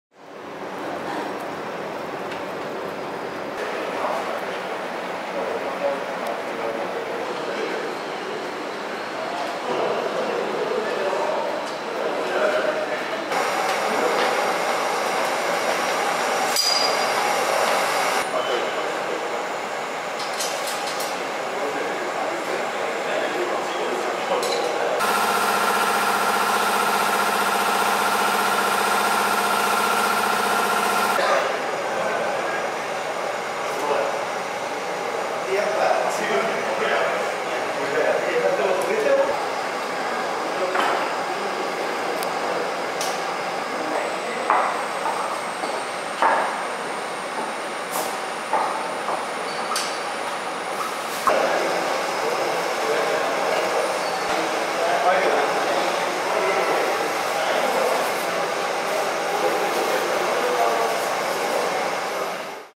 FⅠ松阪市制施行10周年記念・最終日 朝
FⅠ松阪市制施行10周年記念
最終日、朝の検車場の様子です